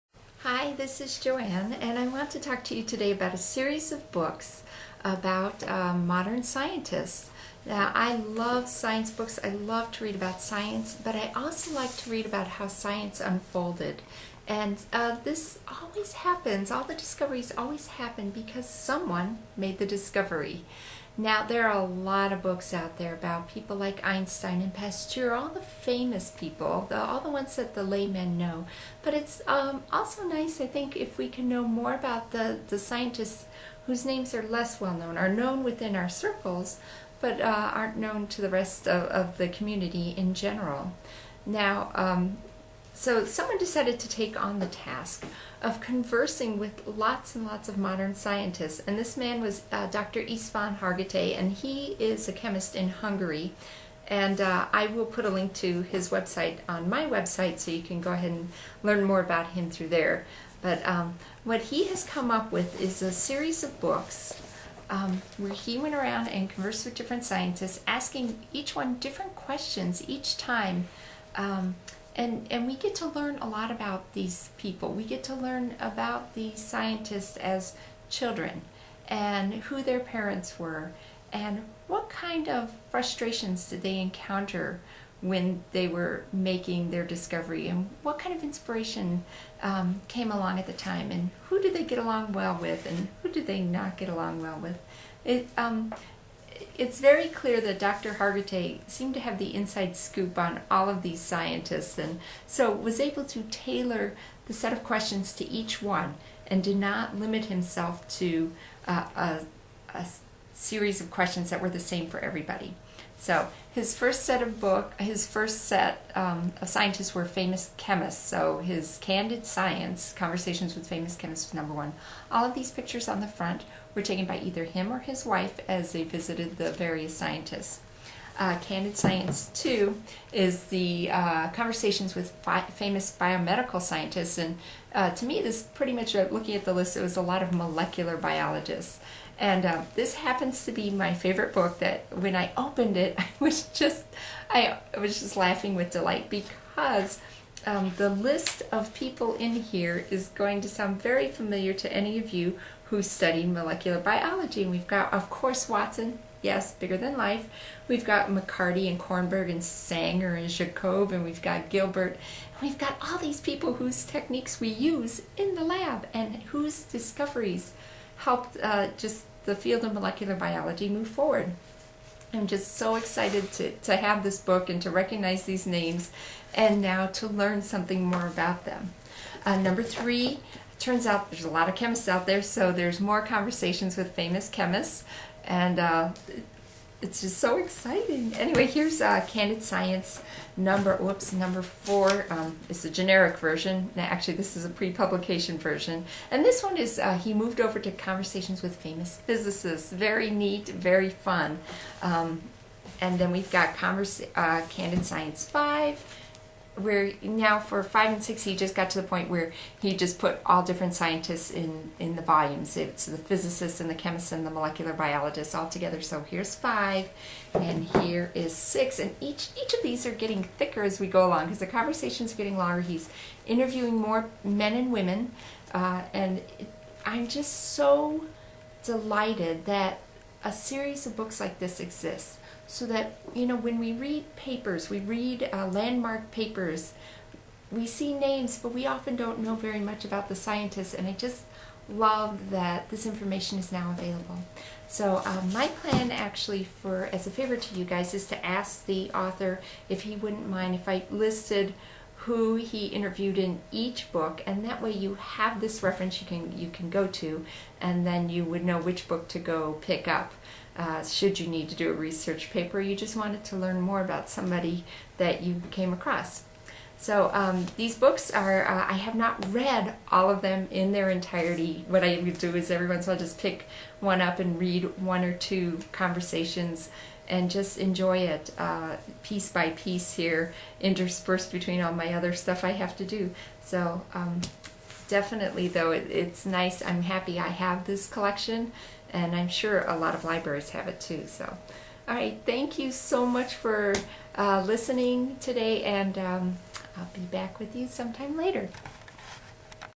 Candid Science review
Joanne highlights the series of books by Istvan Hargittai wherein he converses with numerous modern scientists and records their lives and opinions for posterity.